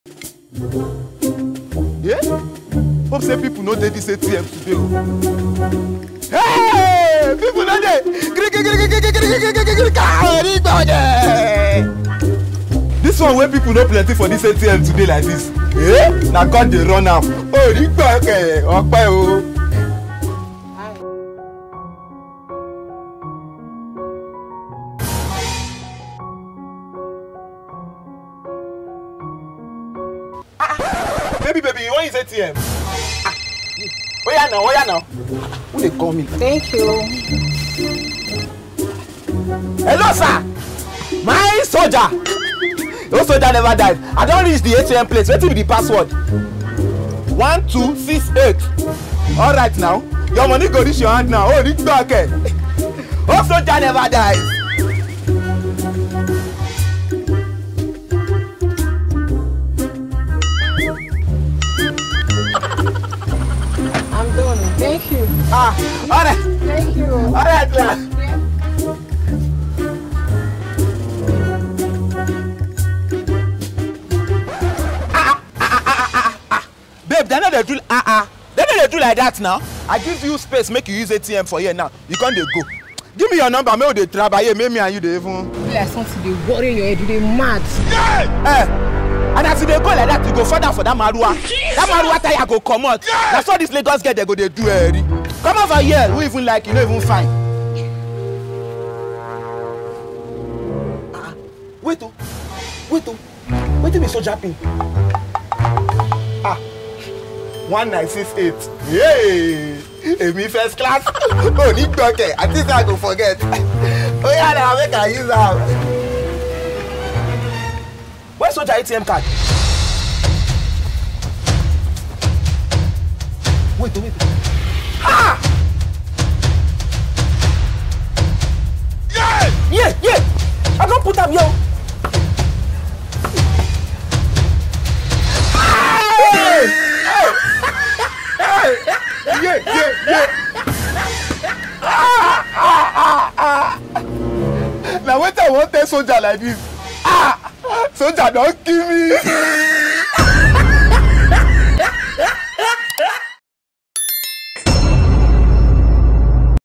THE LADY AT THE ATM
strongperson meets a fine girl at the atm, but while trying to get her number, watch what happened😂😂😂. featuring sophiat